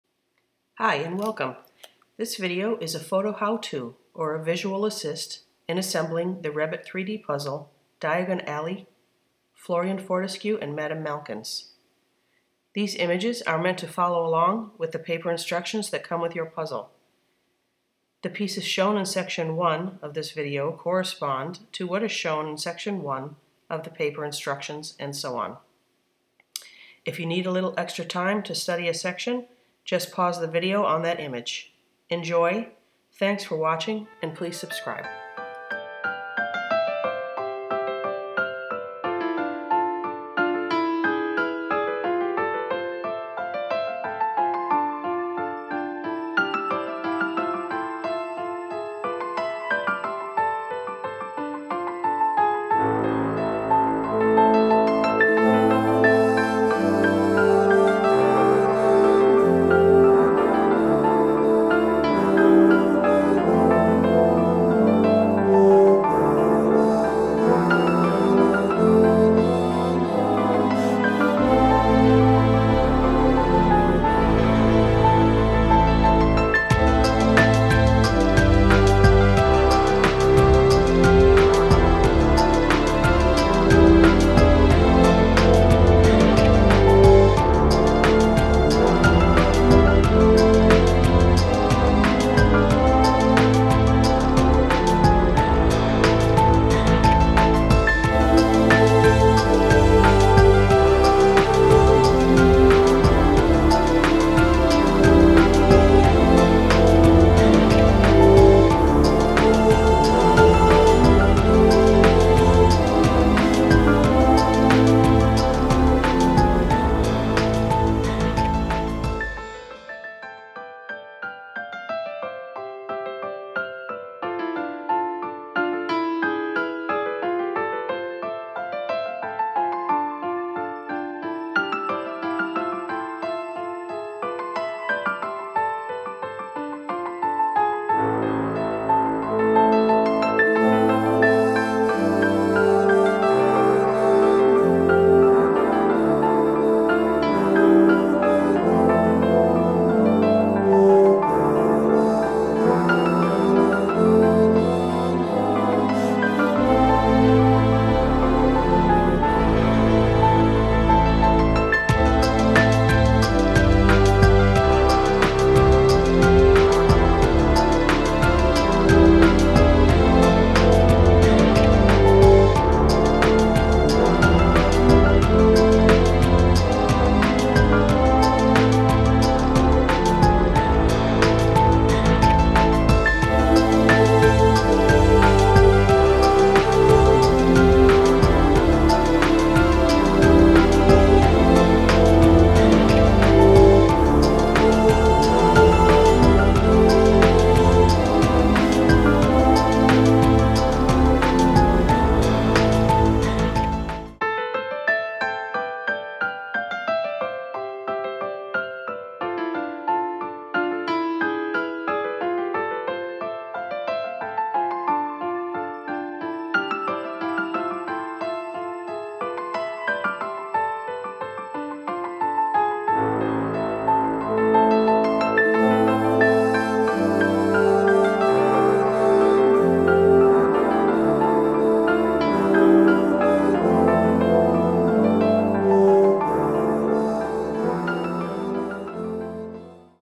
Harry Potter Wrebbit 3D Puzzle • Diagon Alley Collection • Florean Fortescue & Madam Malkin's
Hello puzzlers! If you're trying out your first 3D puzzle or just need a helping hand with this one, this video will let you see each section of the puzzle as it progresses through to completion. This is one of the Wrebbit 3D Puzzles Diagon Alley collection from the Harry Potter movies. Florean Fortescue & Madam Malkin's is one of four from this collection. Have fun and enjoy!